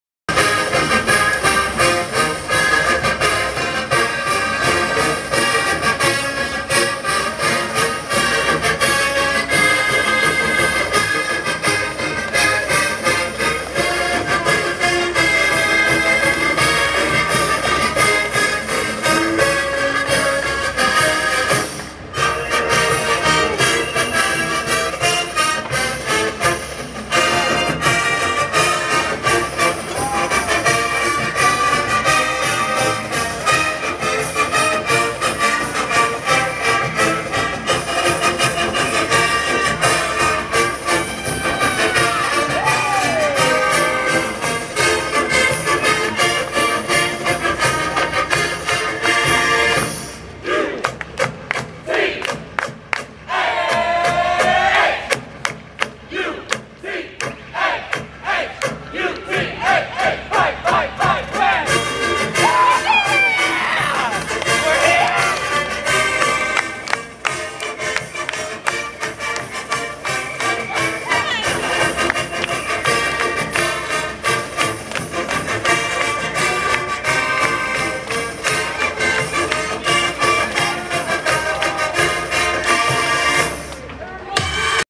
Utah Spirit Team PAC-12 Tournament Fan Fest 3/11/2020
Utah Spirit Team performing at PAC-12 Tournament Fan Fest at T-Mobile Arena in Las Vegas before Utah Utes vs Oregon State Beavers basketball game 3/11/2020

Utah Cheer, Utah Cheerleaders, Utah Dance Team, Utah Utes, Utah Spirit Squad